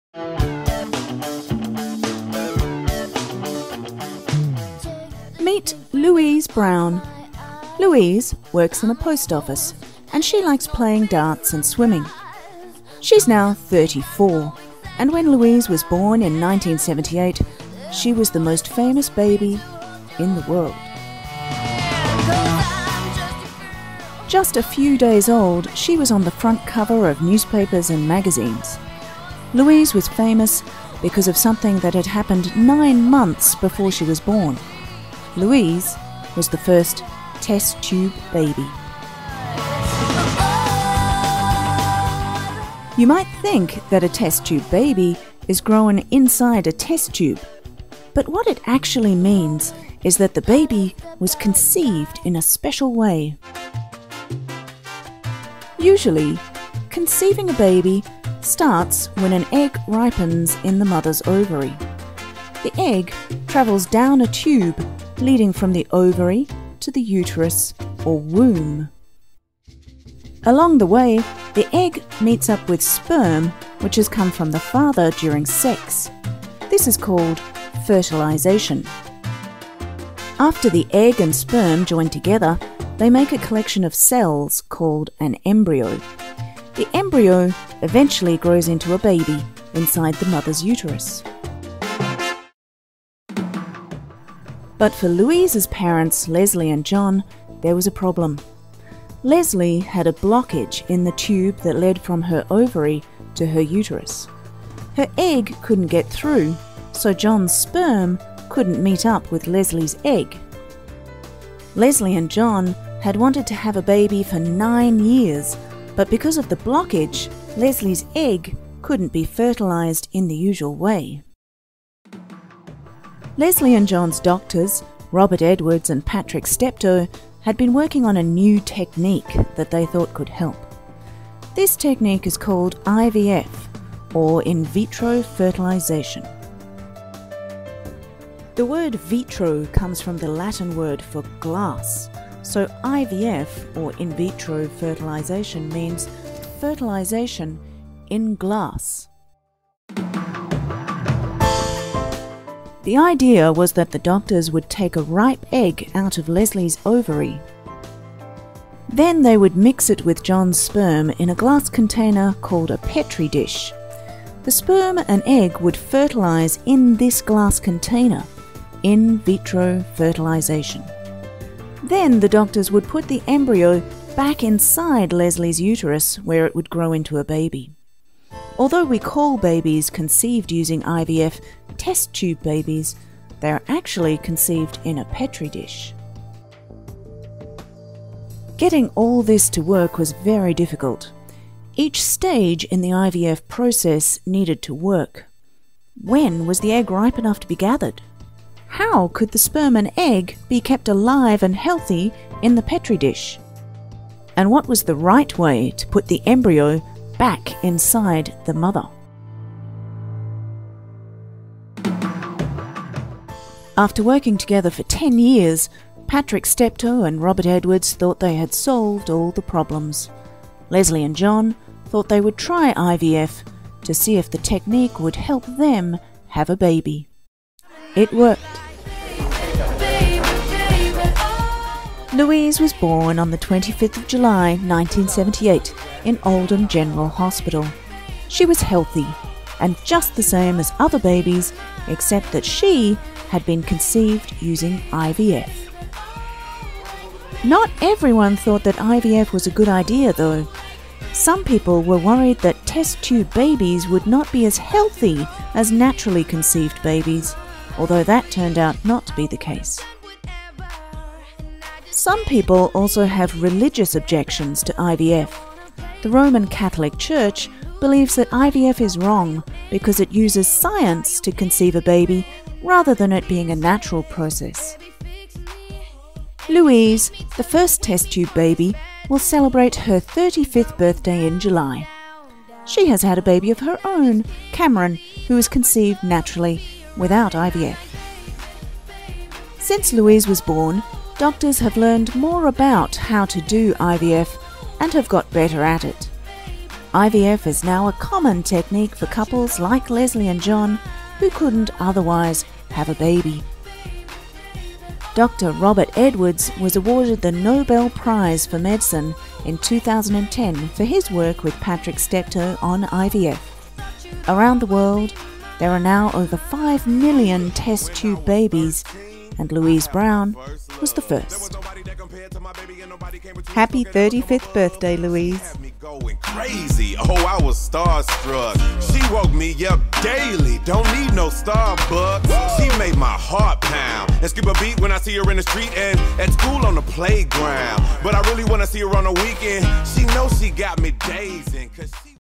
Louise Brown - The First Test Tube Baby
A short documentary for 12-13-year-olds on Louise Brown, the first test tube baby, and the history of in vitro fertilisation (IVF).